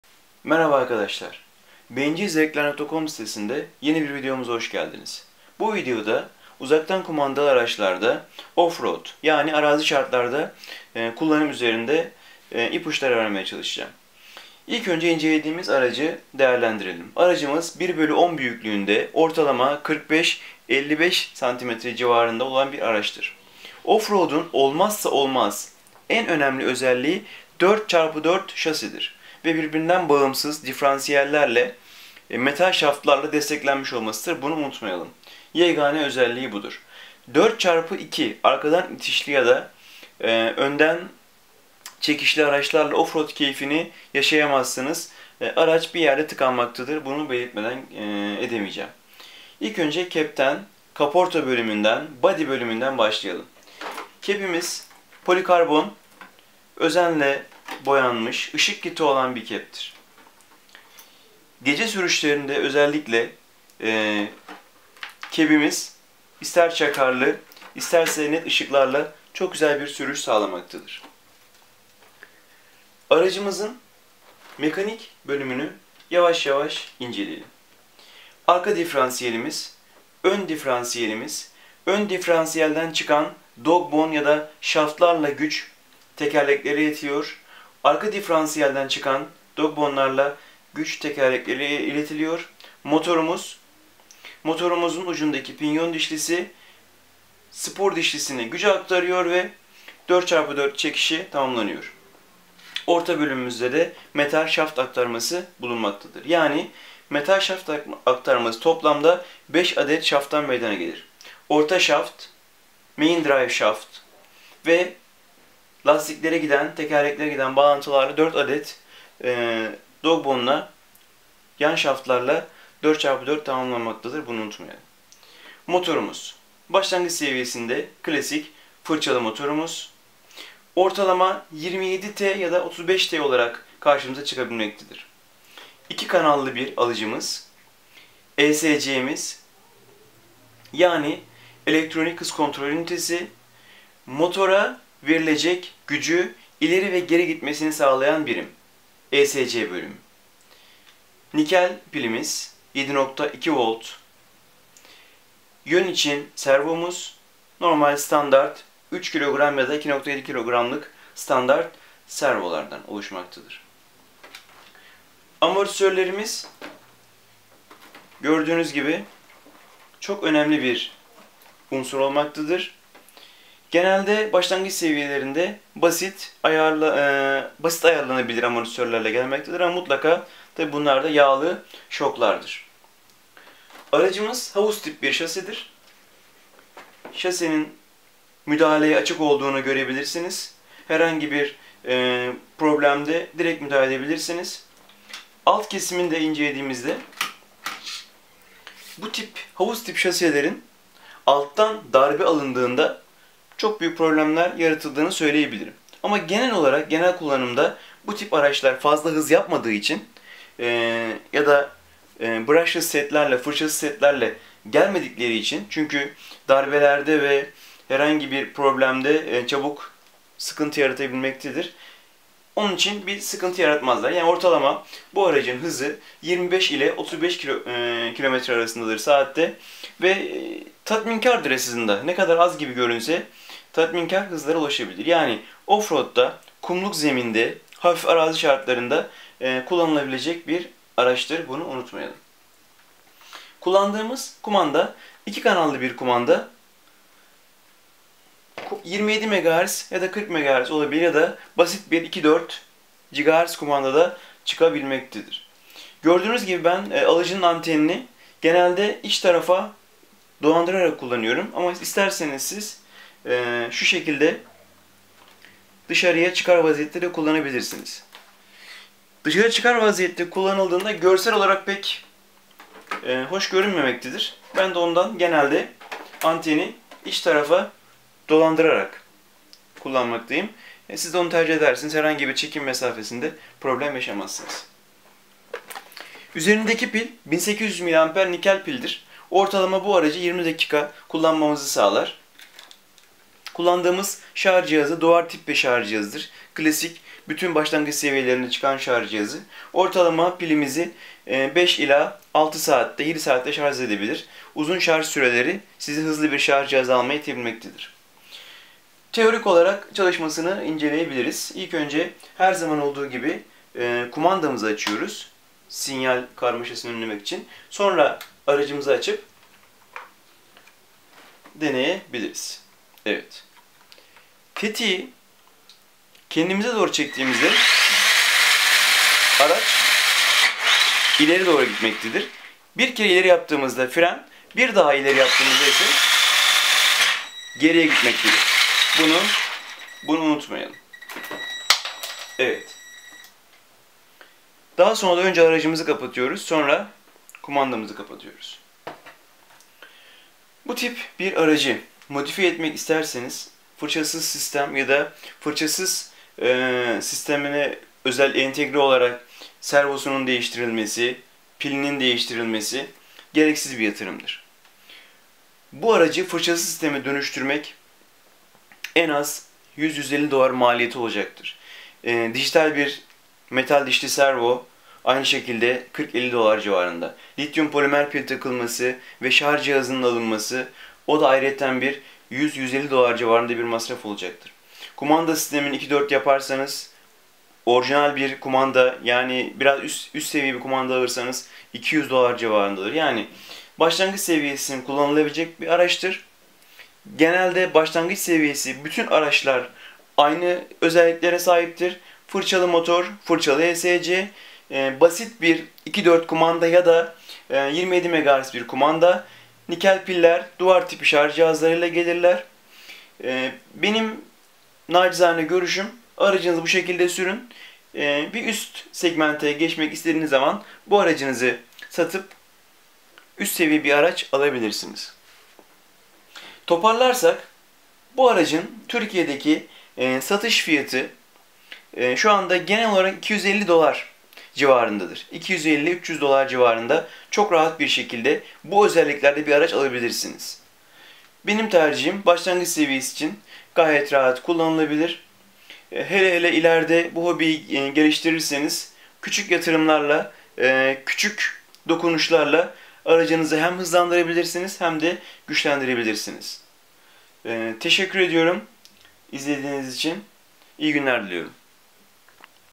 Oyuncak RC KAMYON 1/10 RC Uzaktan kumandalı kamyon
:)😊💕 Rc uzaktan kumandalı 1/10 kamyon incelemesidir. MT ya da monster truck dense de siz CANAVAR KAMYON DEYİN :)
Başlangıç seviye araç genelde aynı özelliklere sahiptir.
Daha fazla içerik için sitemi ziyaret edebilirsiniz.
saygılar.